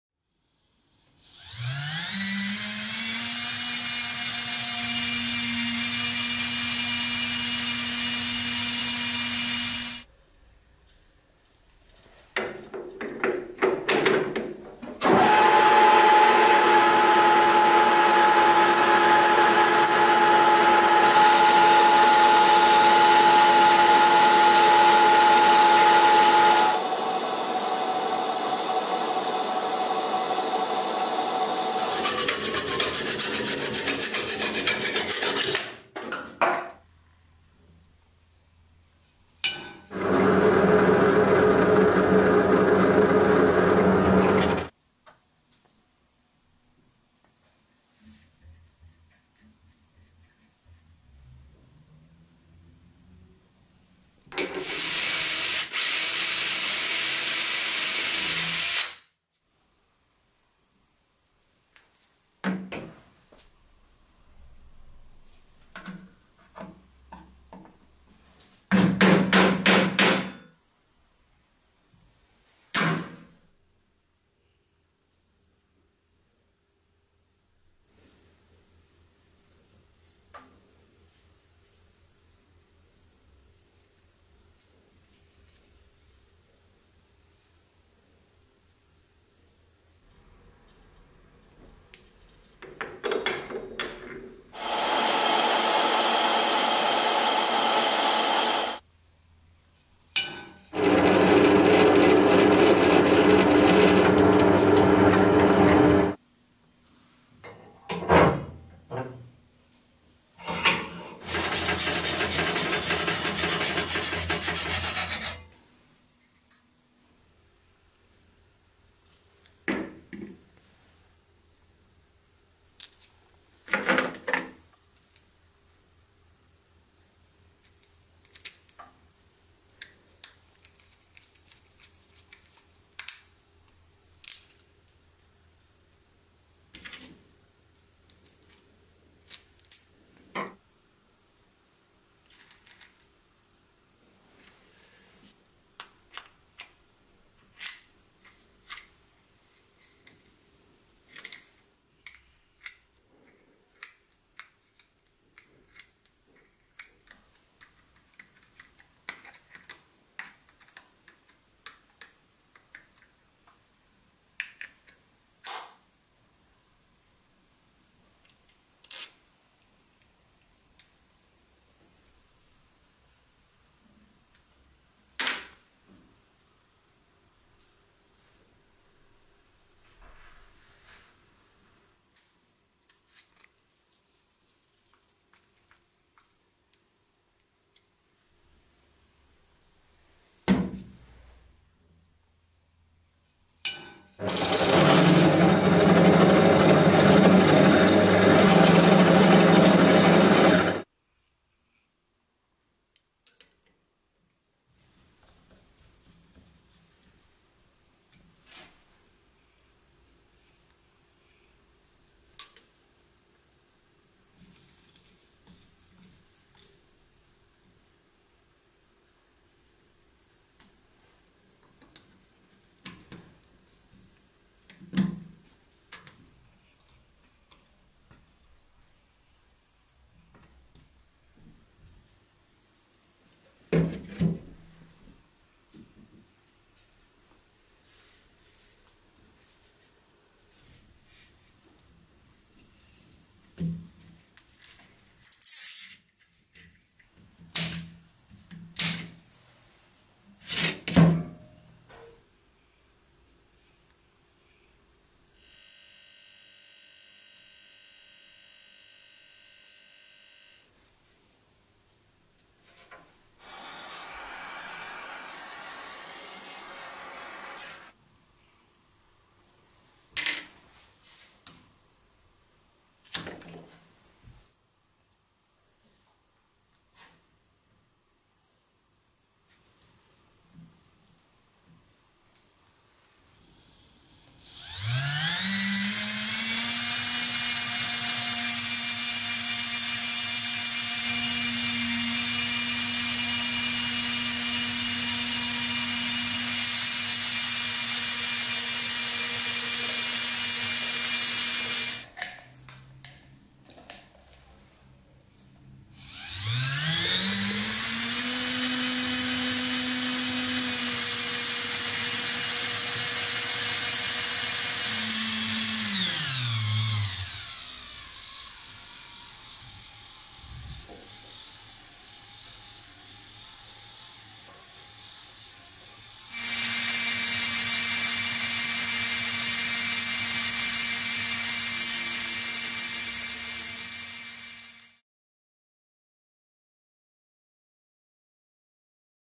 Making a Simple Peristaltic Pump
Making of a simple pump for liquid or air, made of aluminum